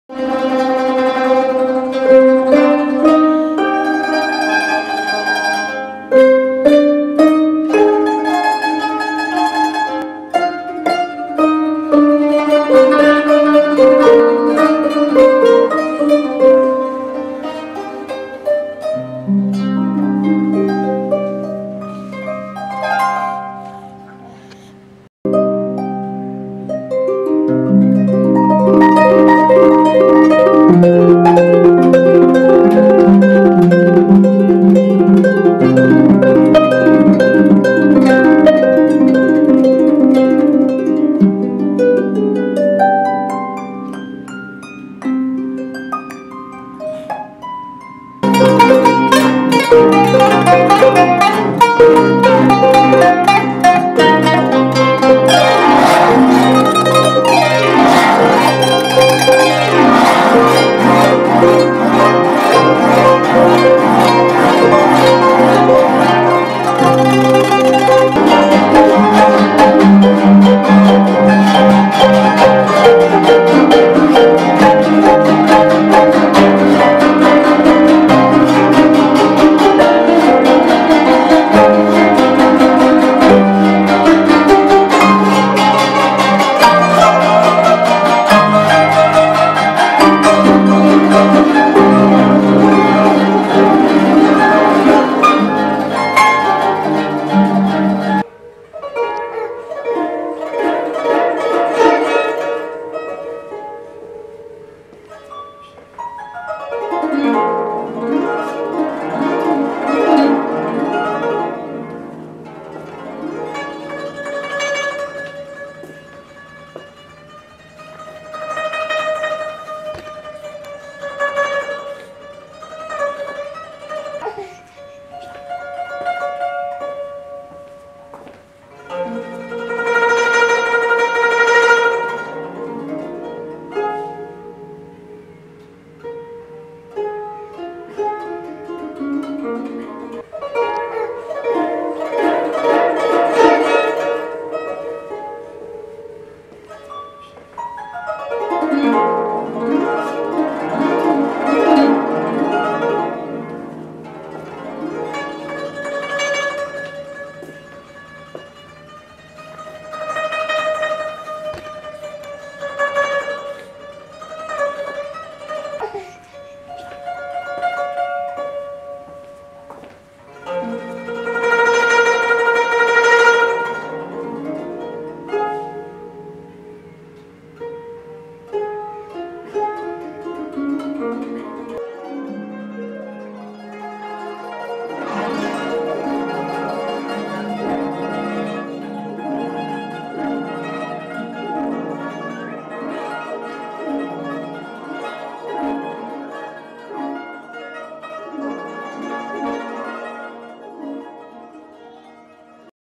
Répétition Cithare du monde
Hend ZOUARI (cithare orientale), Bing Wu-Lamagnère (Guzheng) et Isabelle Olivier (harpe) nous emmène pour un voyage musicale poétique alliant improvisation, classicisme pour un échange musical à la lisière des continents.